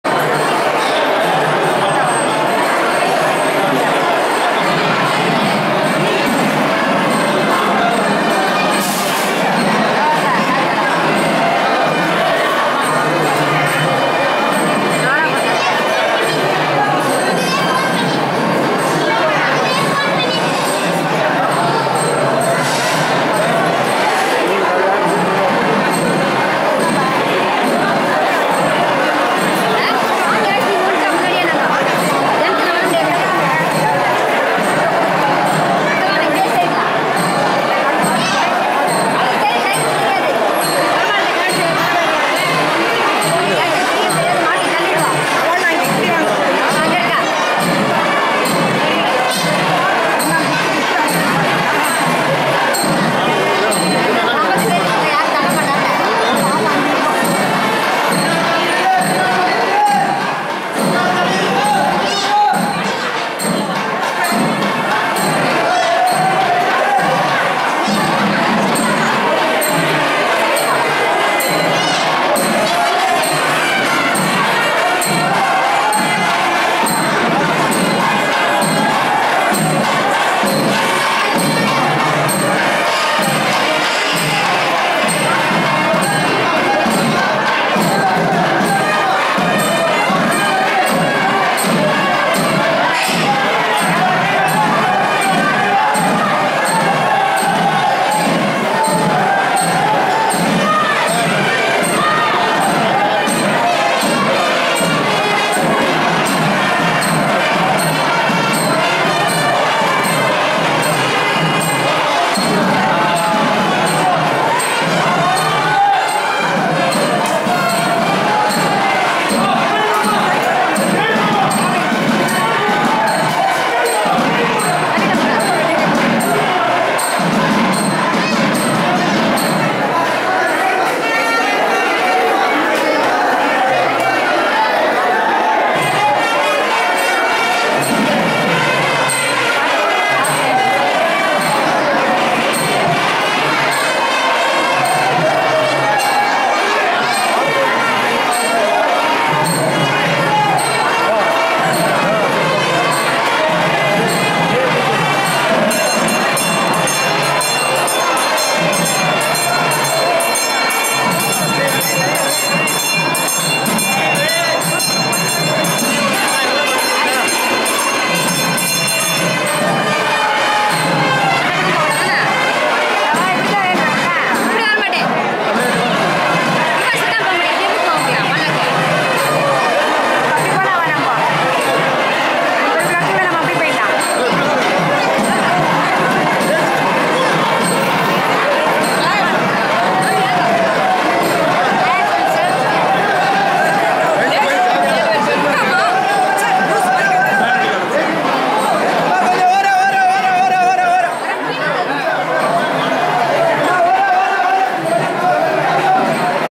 Exclusive sevai to KANAKAVALLI Thayar in GARUDA VAHANAM on 28/4/12 @ Thayar sannithy00126.MTS
At Tiruvallur on Garudasevai utsavam day, Lord VEERARAGHAVAN on return, straight away reaches Thayar KANAKAVALLI SANNITHY to give HER special darshan exclusively for HER. Then Swamy DESIKAN also is offered a special sevai in Garuda vahanam.